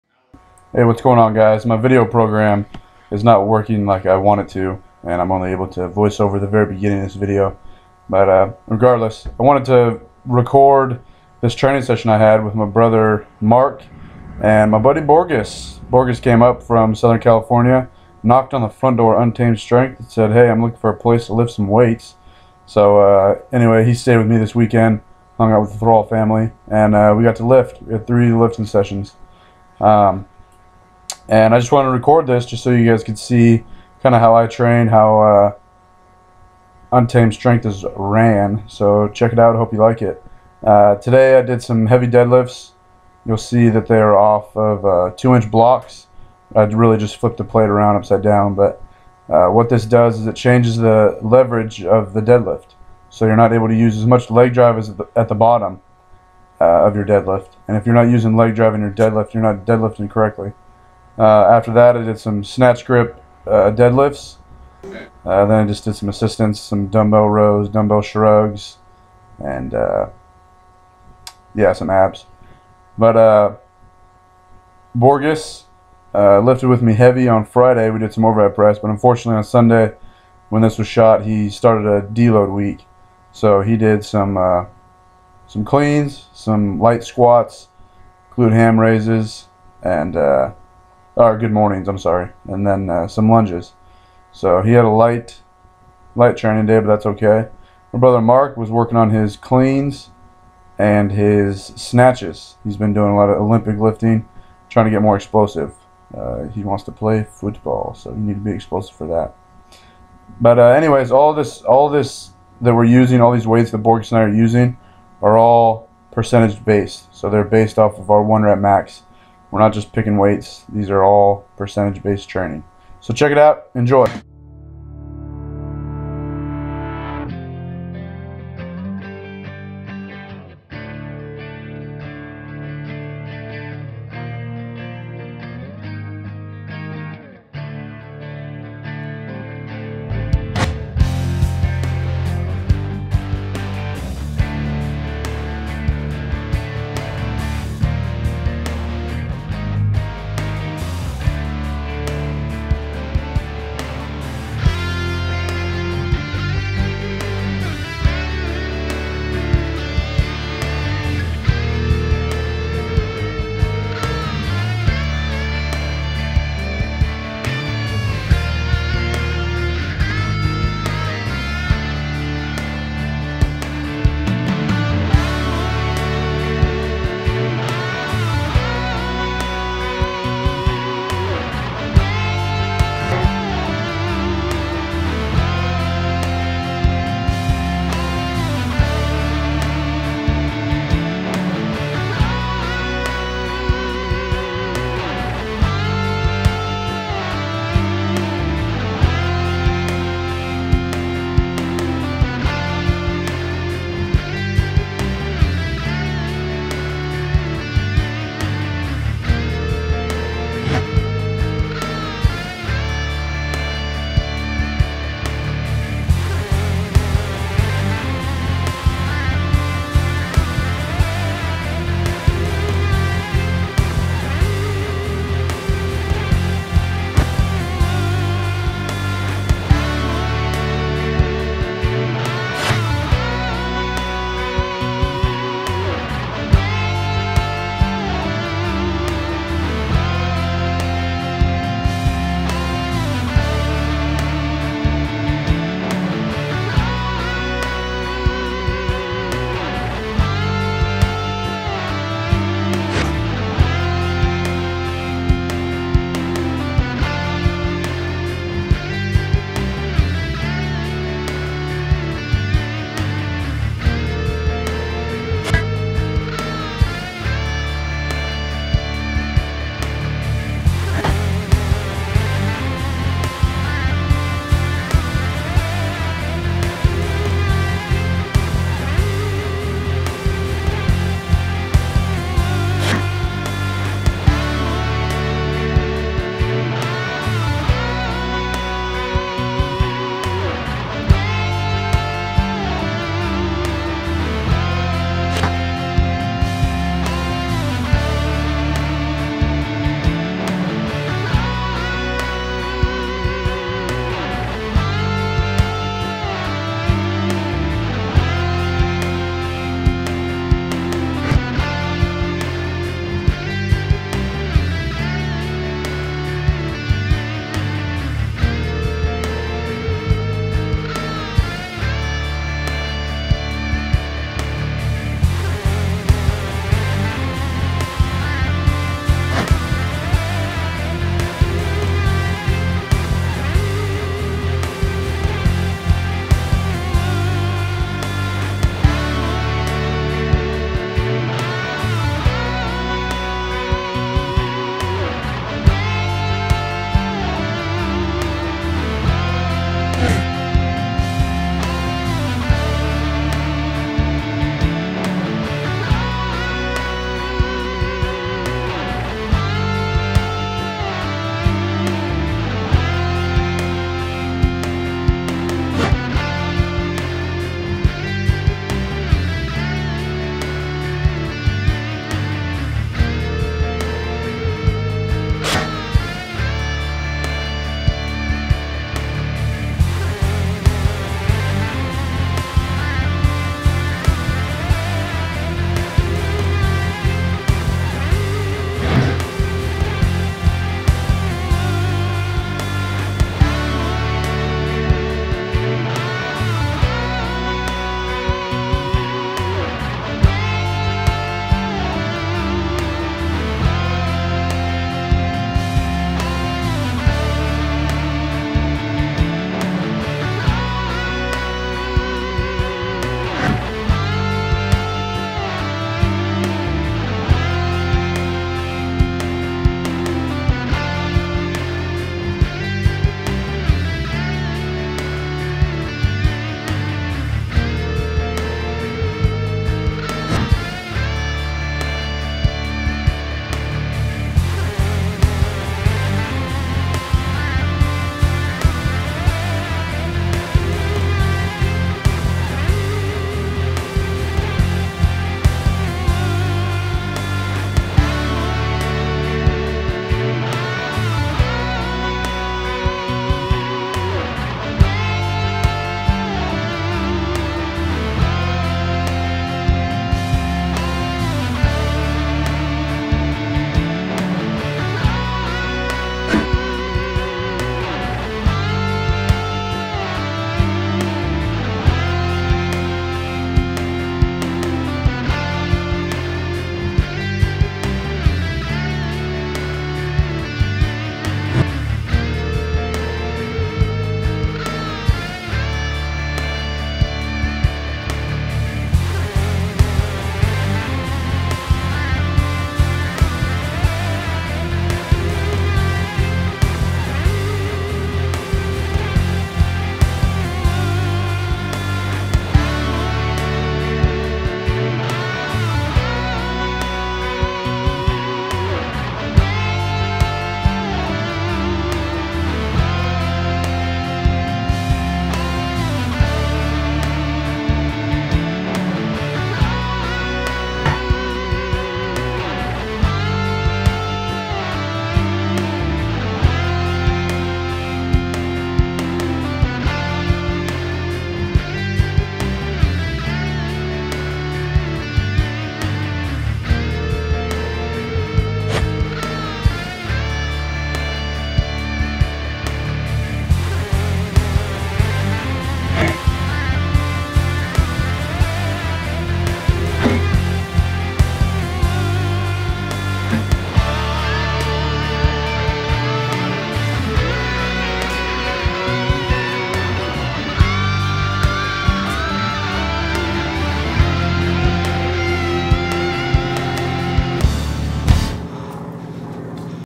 BashinWBorgus
Cleans, Deadlifts of 2" block, snatch grip deadlifts, light squats